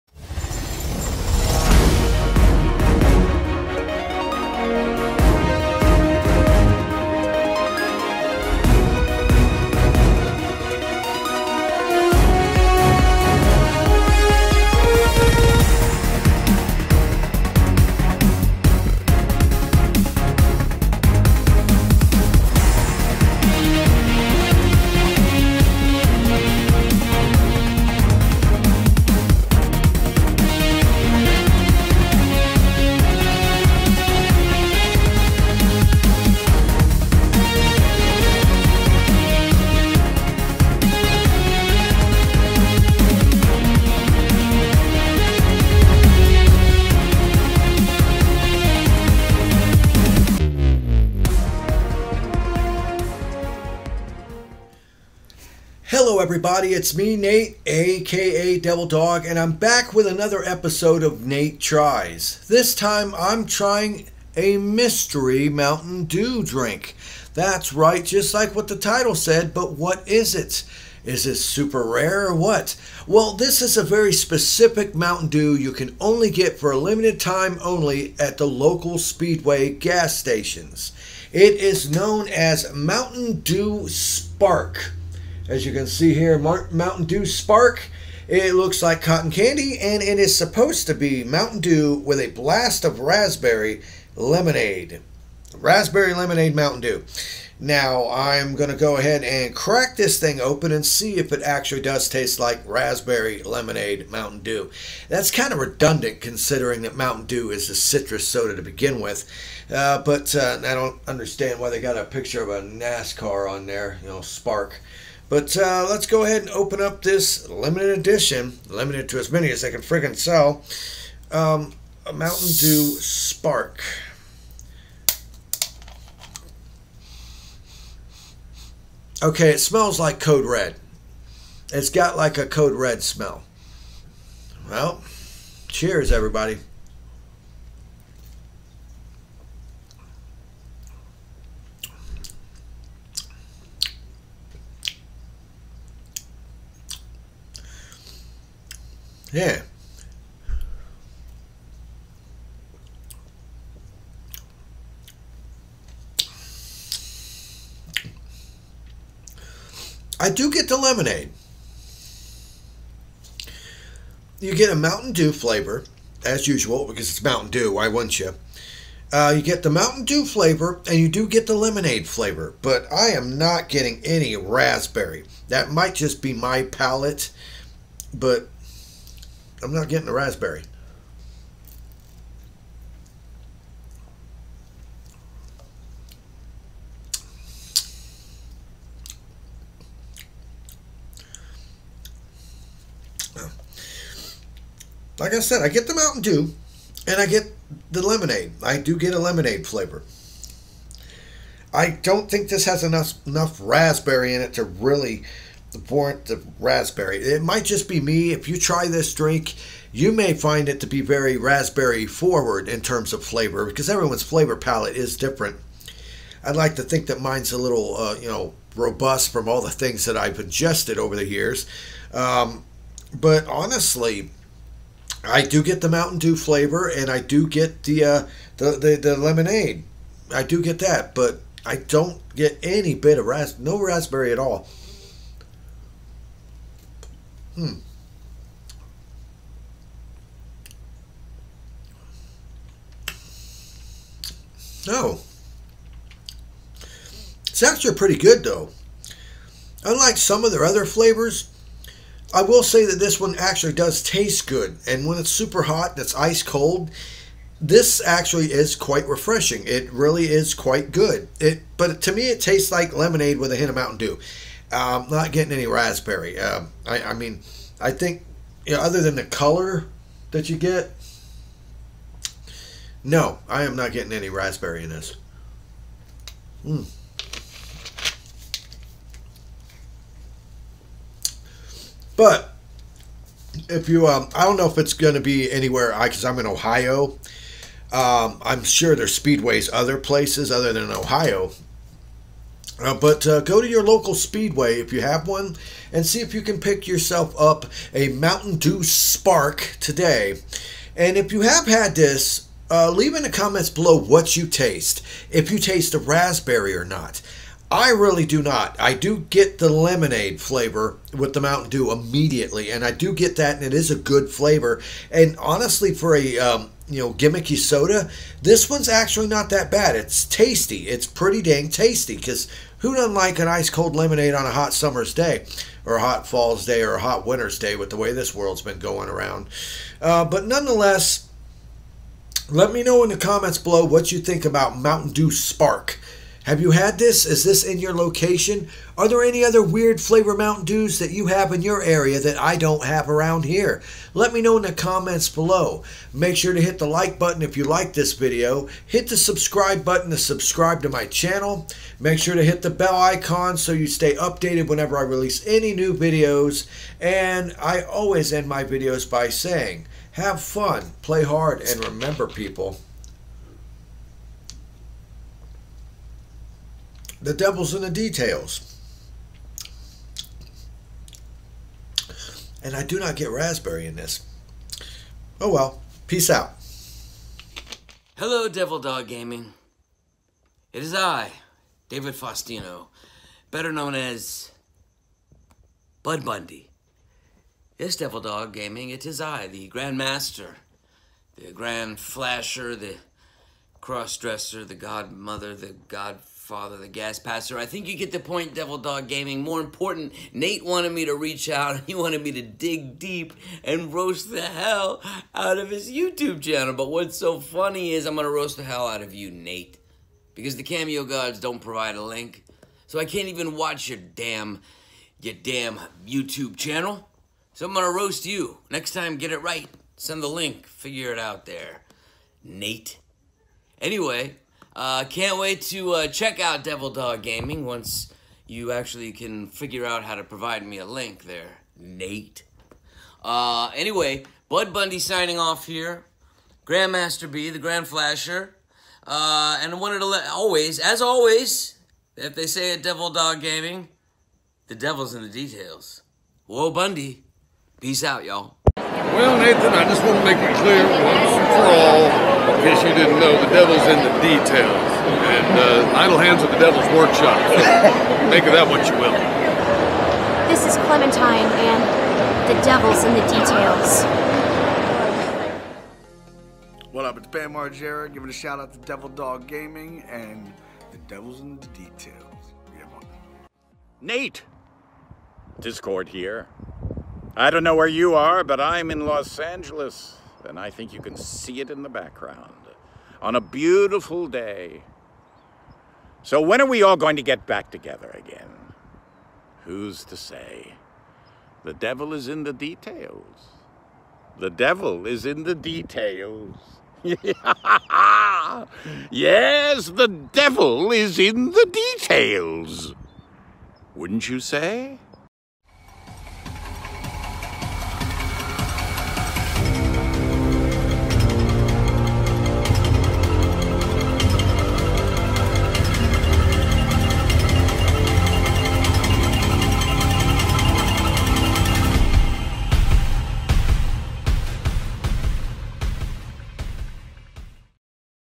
Nate Tries: Mystery Mountain Dew Flavor!!
A new mystery mountain Dew has appeared at a local business... But what is the new mystery Flavor, and where to get it??